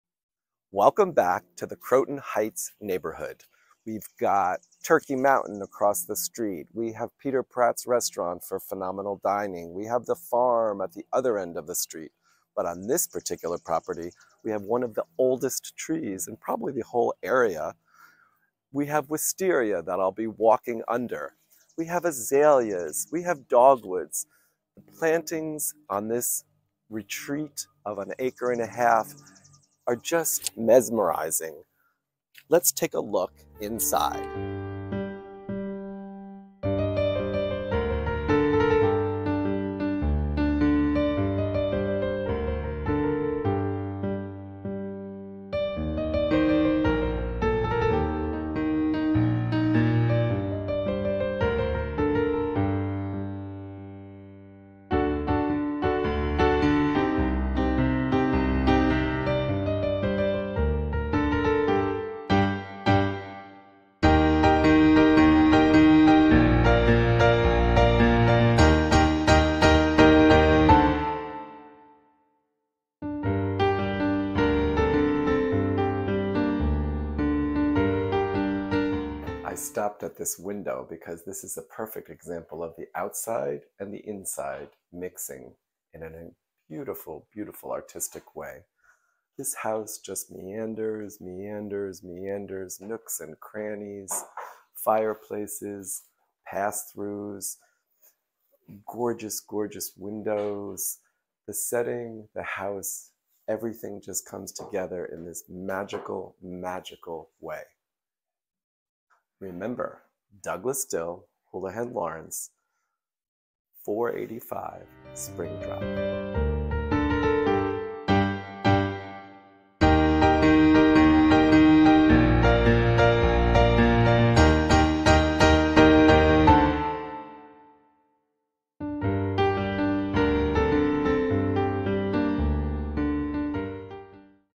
485 SPRING DRIVE YORKTOWN HEIGHTS, NY 10598
This is the latest video for 485 SPRING DRIVE YORKTOWN HEIGHTS, NY 10598 for more information A Croton Heights Charmer. What started as a cottage has been transformed into a wonderful rambling unique home. The magical feel follows from room to room with every turn uncovering yet another whimsical feature. The main floor is delightful with a cozy library, multiple fireplaces, and windows everywhere to take full advantage of the spectacular setting. The primary suite is a three-room fantasy with sitting areas, exposed brick, and limitless storage options. The nooks and crannies with Harry Potter cabinets appeal to all ages. Be only the fourth owner in 100 years. With access to the North County Trailway, Turkey Mountain (the highest point in Westchester), and a quick bike ride into town, the convenience is yours. Walk to Peter Pratts Restaurant, around the corner, for an exquisite dinner al fresco or by the fireplace and stroll home. The neighborhood also boasts Hill Top Hanover Farm for a CSA or just stop in for fresh vegetables in the summer. With one of the oldest neighborhood associations in the area, the feeling of community is very evident. Easy commute to NYC by either car or train, be at Grand Central in an hour.